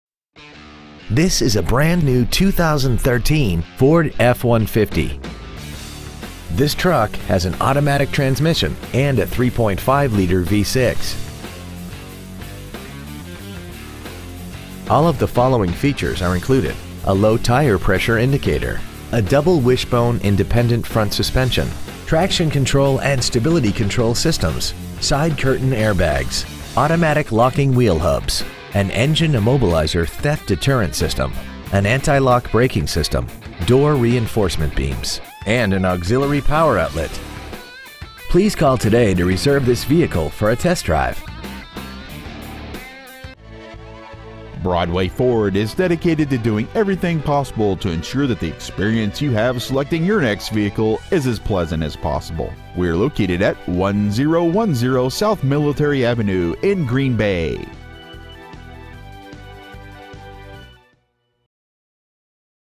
2013 Ford F-150 Green Bay WI
We are proud to present this 2013 Ford F-150 . We have been honored to serve the Green Bay WI area , we promise that your experience at our dealership will exceed your expectations ! Year : 2013 Make : Ford Model : F-150 Engine : 3.5L V6 24V GDI DOHC TWIN TURBO Trans .